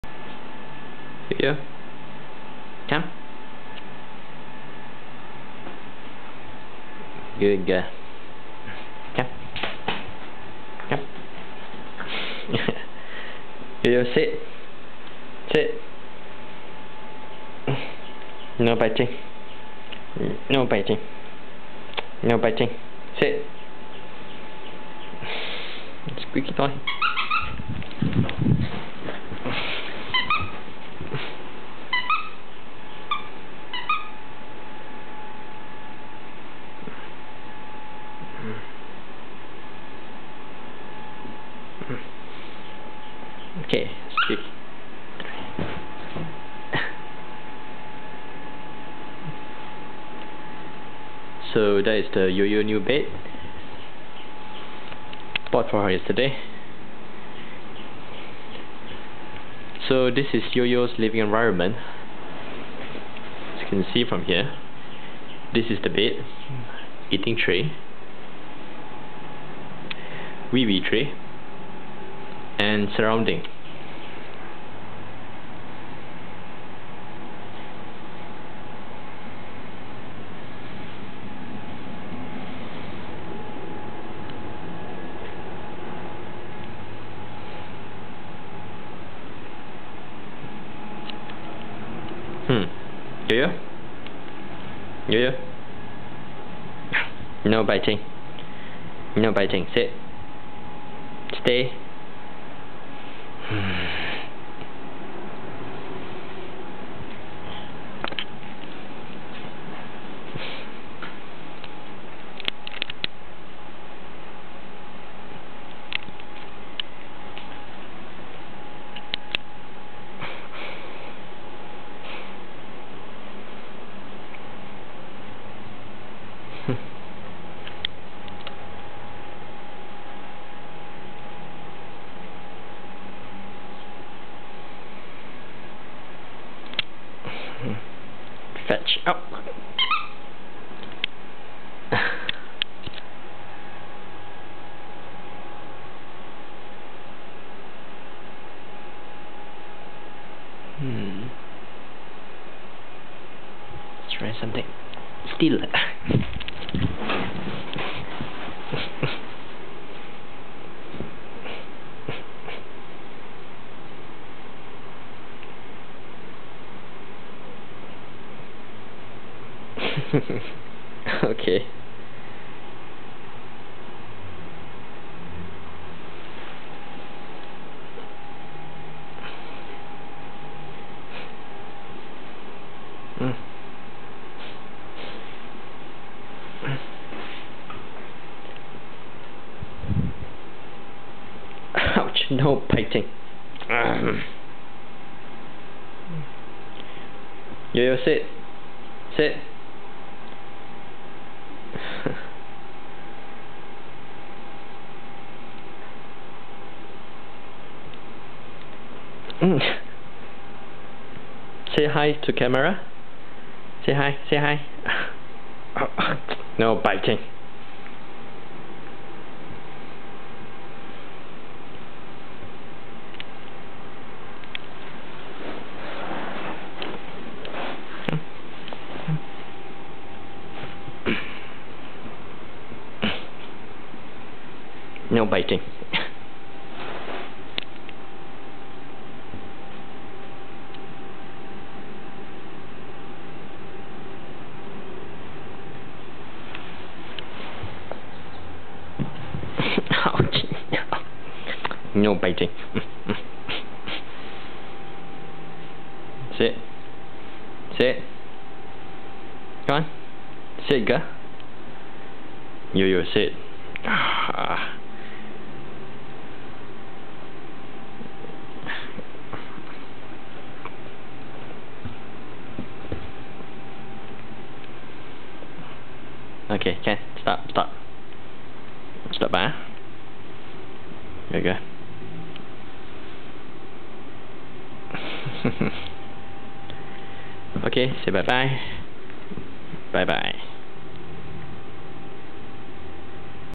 Toy Poodle. Yoyo (act 2)
Recorded my toy poodle playing around! Also introducing her equipments.